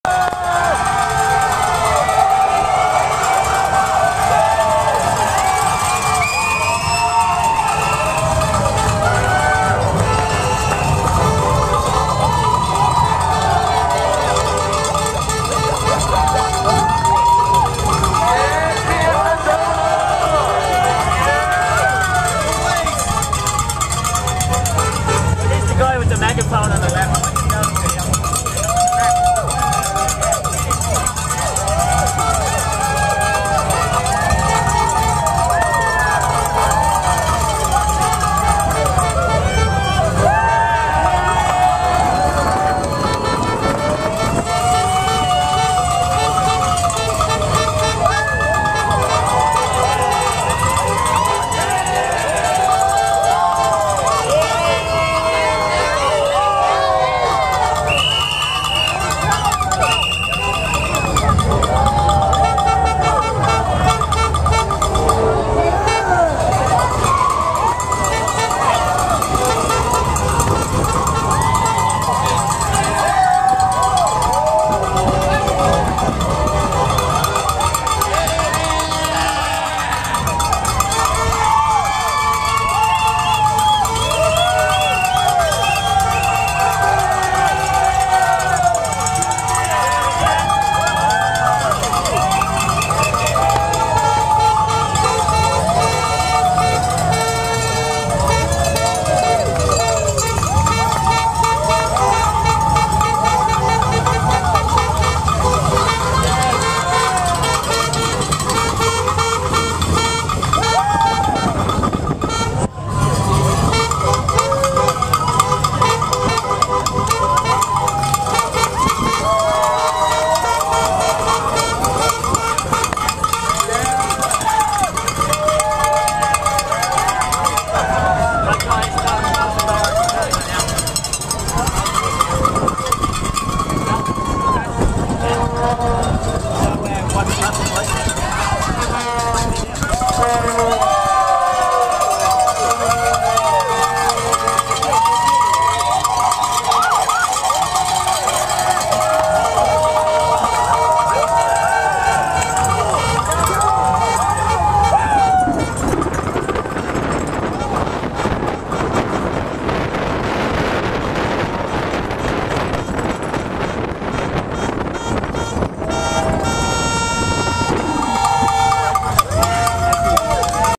The Wall in Manayunk
A ride in a support vehicle up the famous Wall in Manayunk.  Experience the party and mayhem.